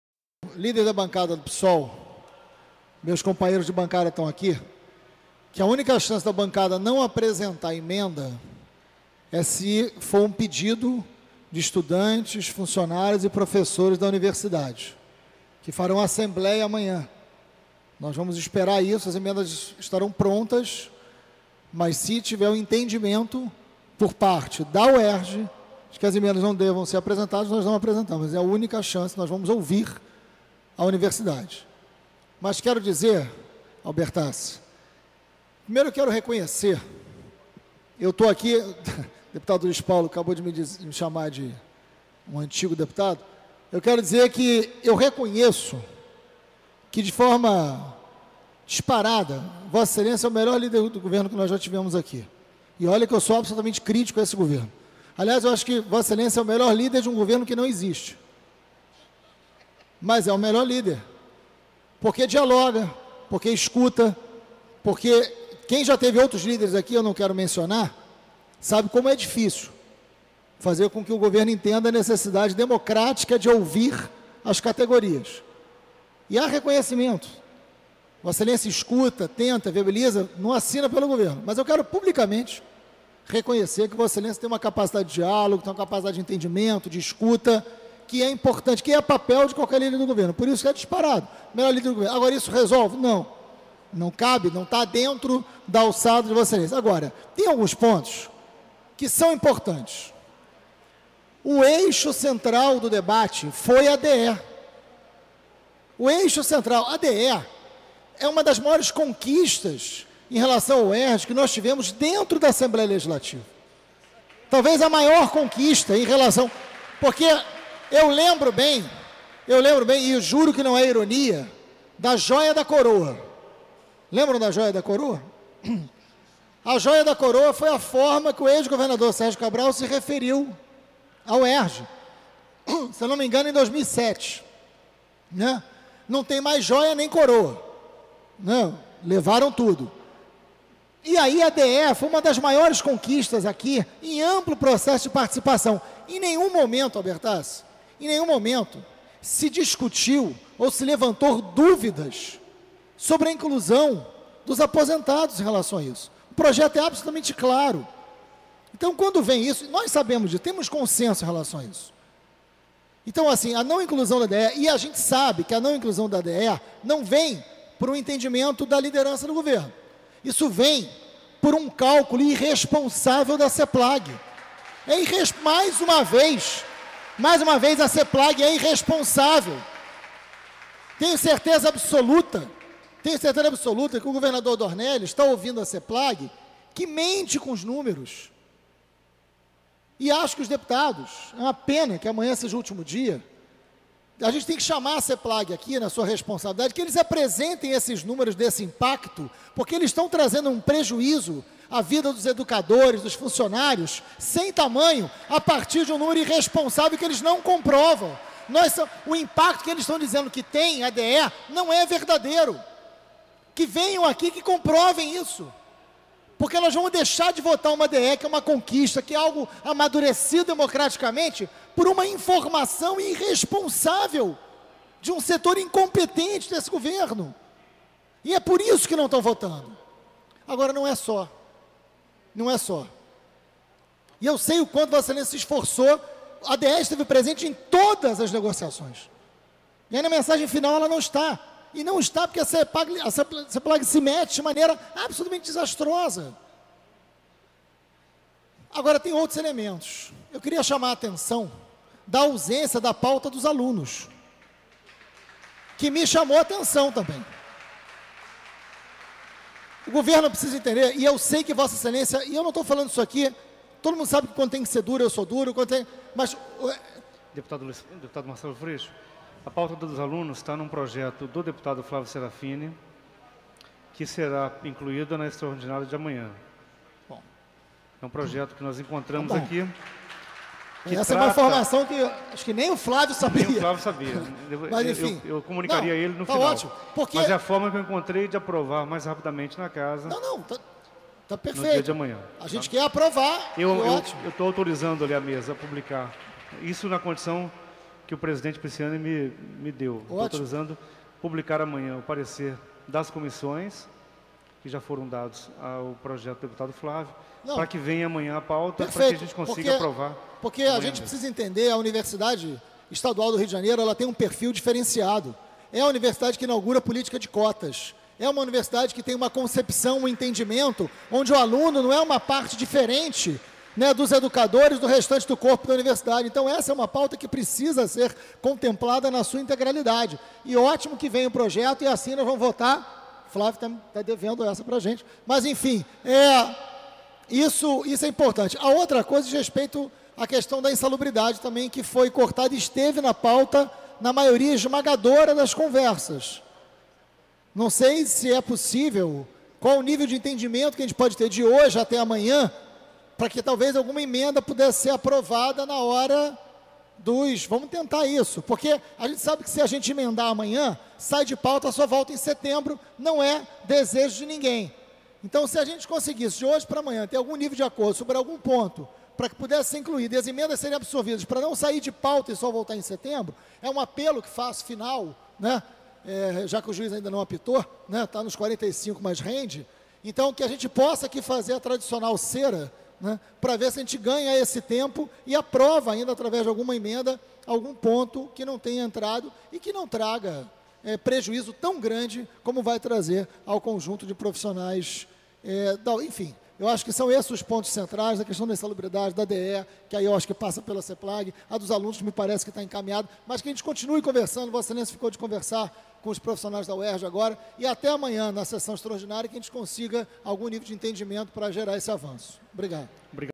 A Alerj tem que votar a Dedicação Exclusiva da Uerj!
A inclusão da Dedicação Exclusiva ao vencimento básico foi uma das maiores conquistas da Uerj que tivemos dentro da Alerj, em amplo processo de participação. A não inclusão da Dedicação Exclusiva vem de um cálculo irresponsável da Seplag.. Temos que chamar a Seplag para eles explicarem os números deste impacto, porque eles estão trazendo um prejuízo sem tamanho para a vida dos educadores e funcionários. Não vamos deixar de votar a Dedicação Exclusiva por uma informação irresponsável. A DE está presente em todas as negociações e nesta ela não está”, afirmou Marcelo Freixo no plenário da Alerj nesta quarta-feira (28)